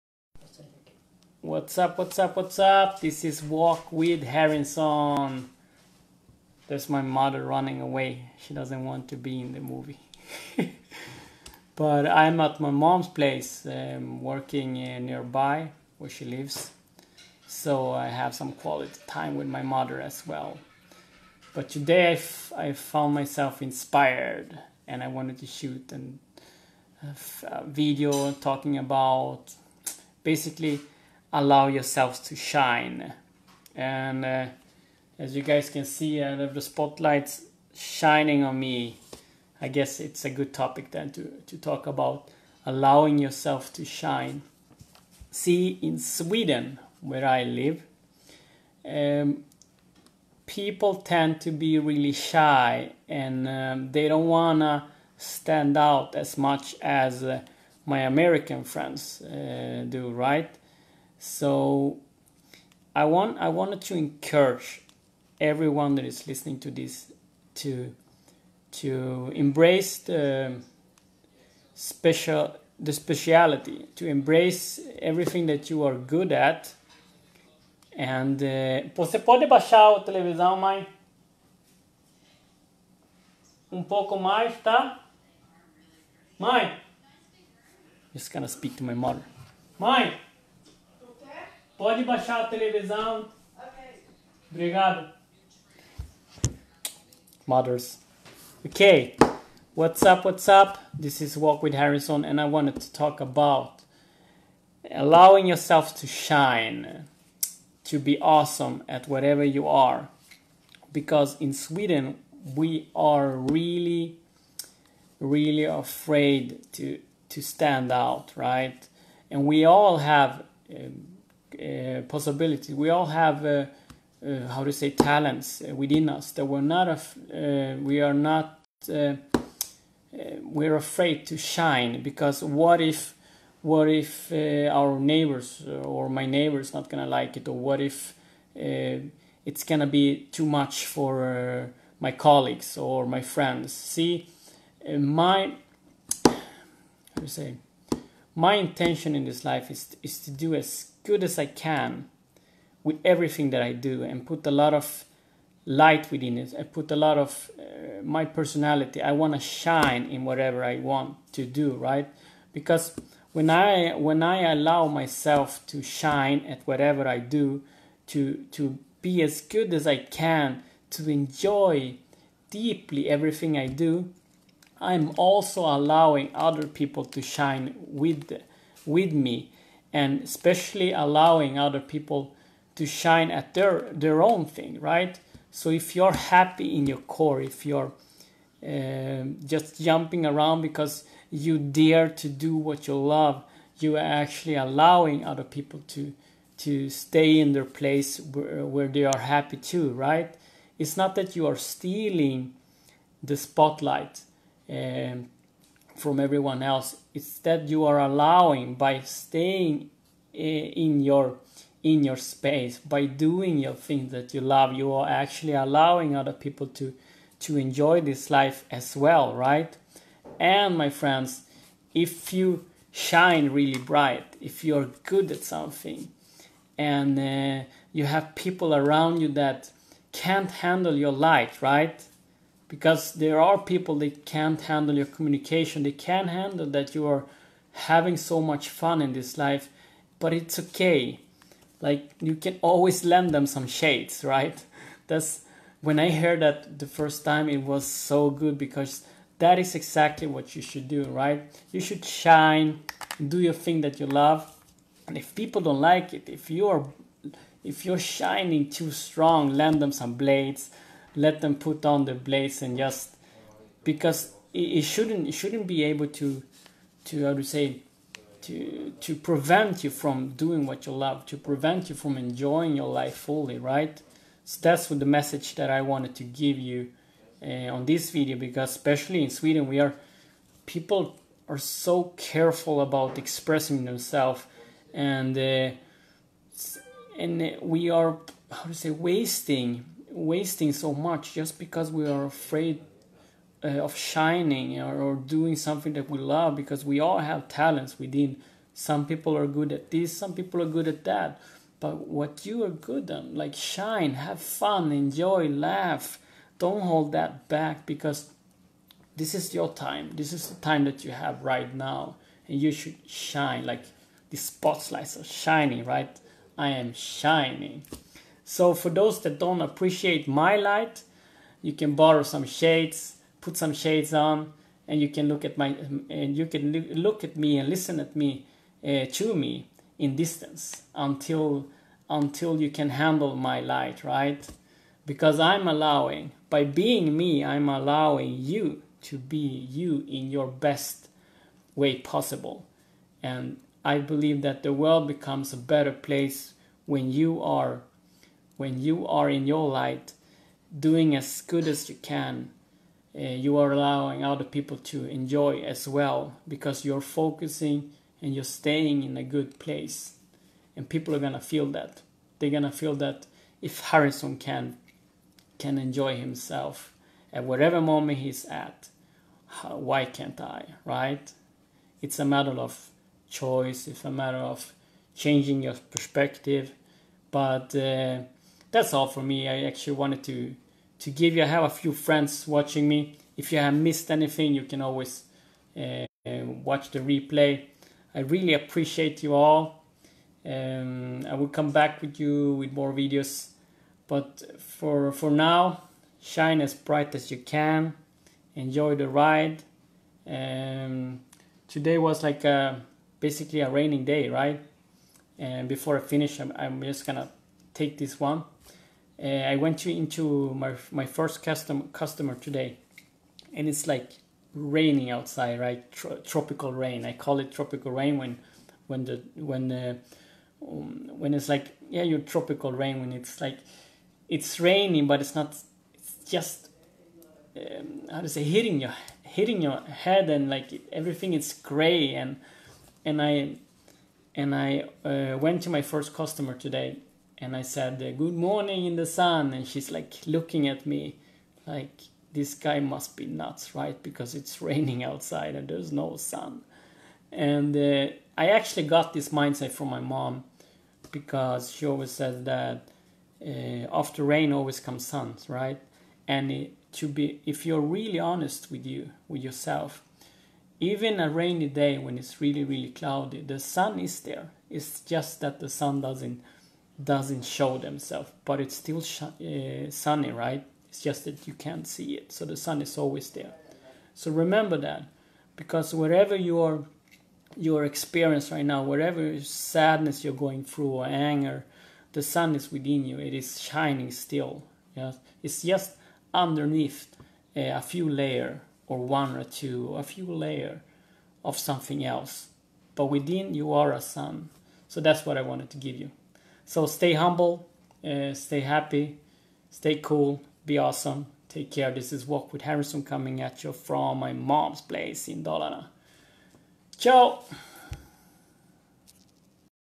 Allow Yourself To Shine!
You're unique just the way you are, others may want to drag you down on a poor path of disbelief. But your greatness is in the way you shine like only you can. BELIEVE in yourself. I do! Allow yourself to shine!